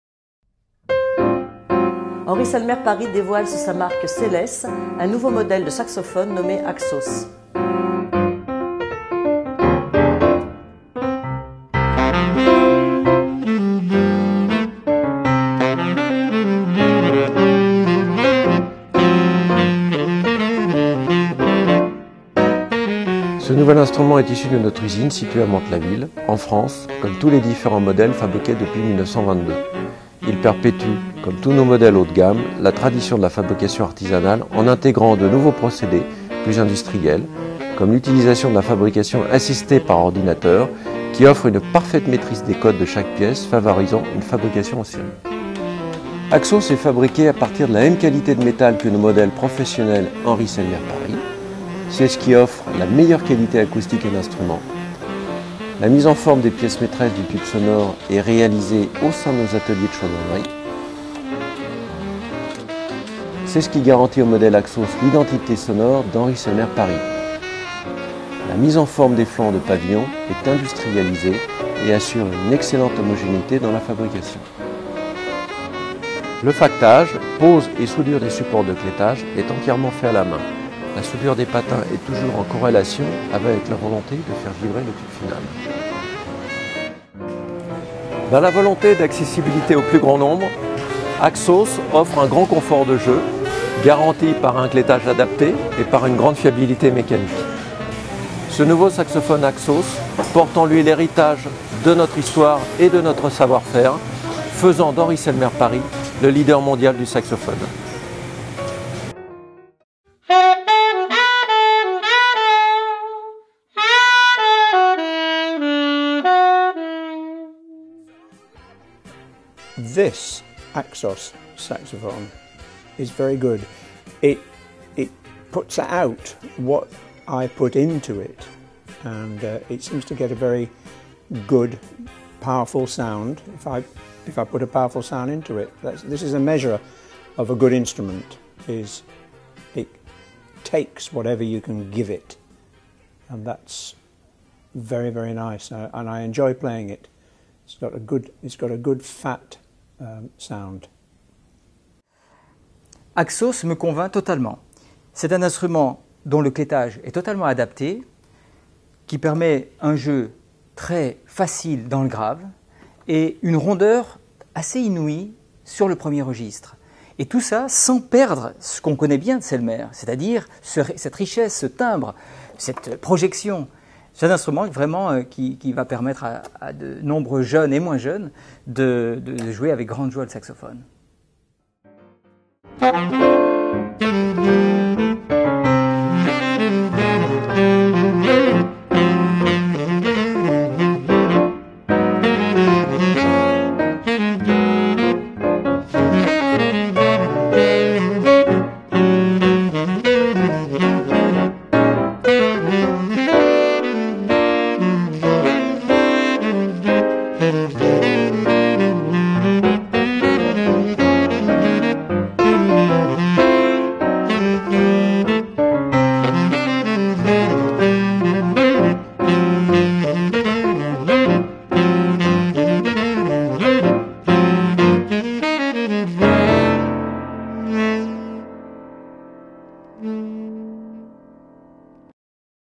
Axos, the new alto sax by SeleS - Henri SELMER Paris
Henri SELMER Paris unveils, with its new brand SeleS, a never before seen saxophone called Axos.
This new saxophone Axos brings with it the heritage of over a century of history and technical knowledge from Henri SELMER Paris, the world leader in saxophone design and production.

Henri SELMER Paris dévoile sous la marque SeleS un modèle inédit de saxophone nommé Axos.
Ce nouveau saxophone Axos porte en lui l’héritage de notre histoire et de notre savoir-faire faisant de Henri SELMER Paris le leader mondial du saxophone.